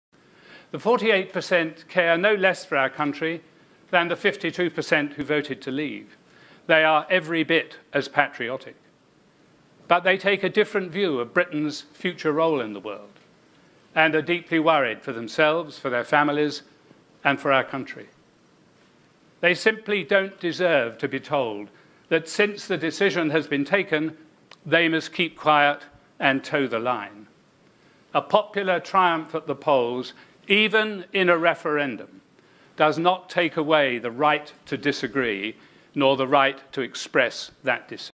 John Major at Chatham House: On Remain and Leave
In his first public speech since the EU referendum, Sir John Major outlined the realities that he believes both Britain and Europe face in the future.